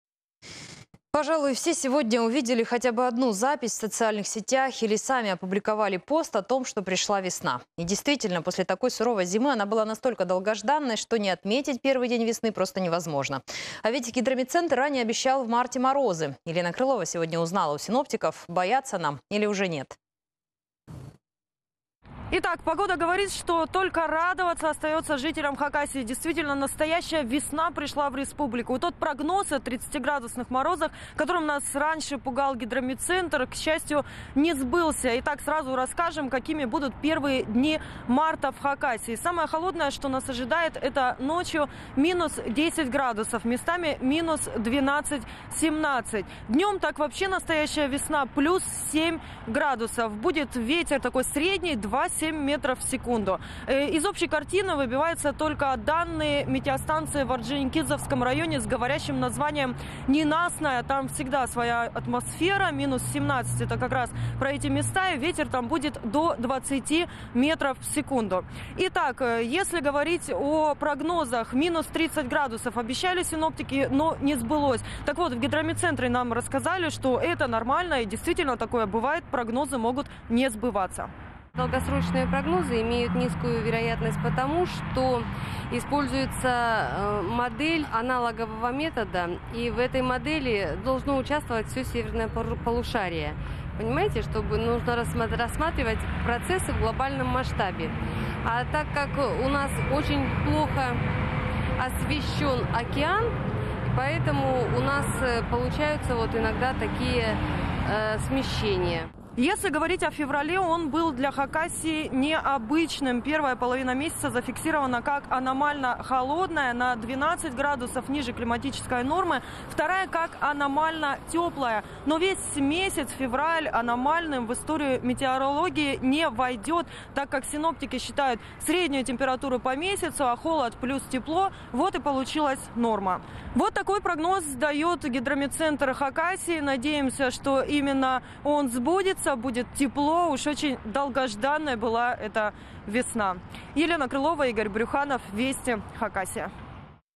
Синоптики составили прогноз на март. 01.03.2019
И действительно после такой суровой зимы она была настолько долгожданной, что не отметить первый день весны просто невозможно. А ведь Гидрометцентр ранее обещал и в марте.